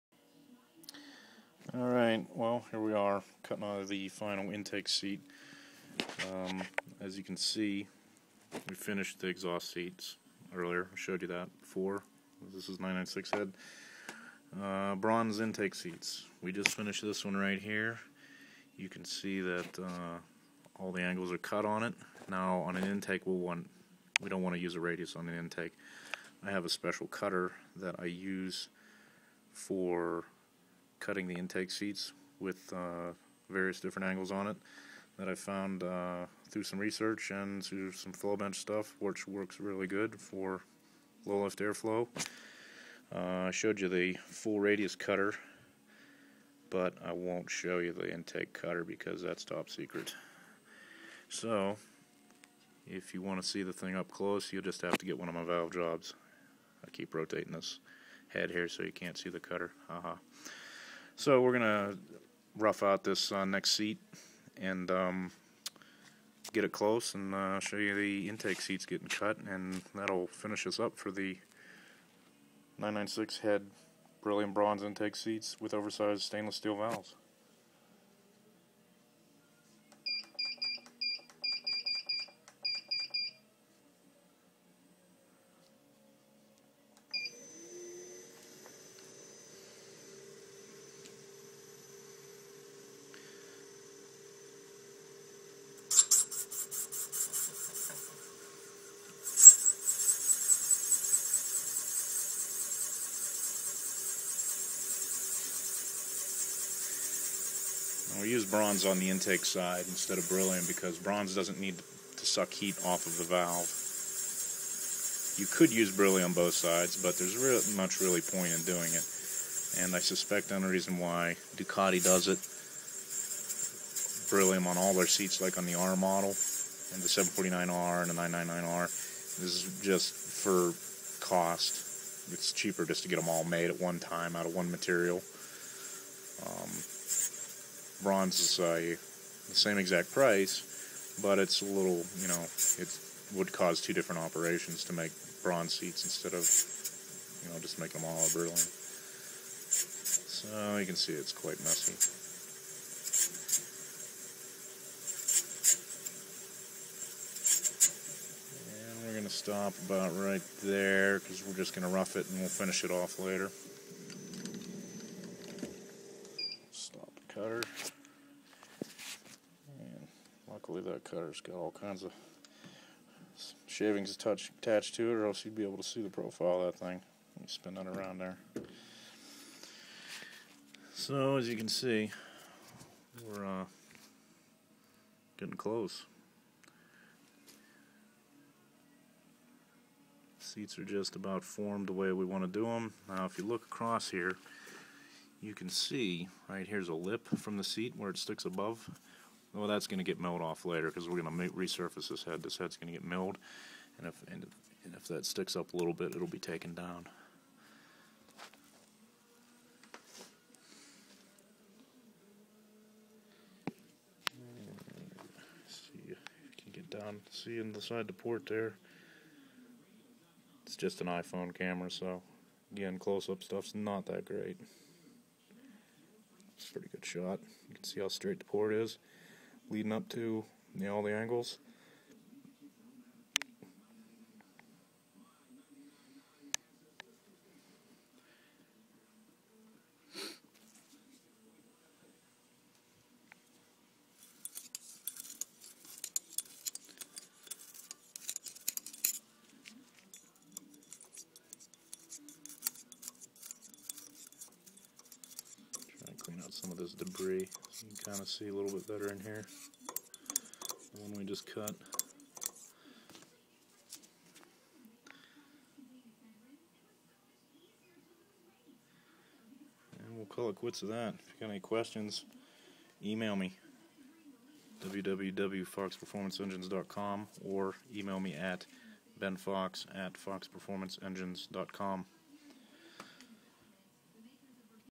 Ducati 996 bronze intake seat cutting
Cutting the final seat angles on the intake seats of a Ducati 996 using bronze seats and oversized stainless valves.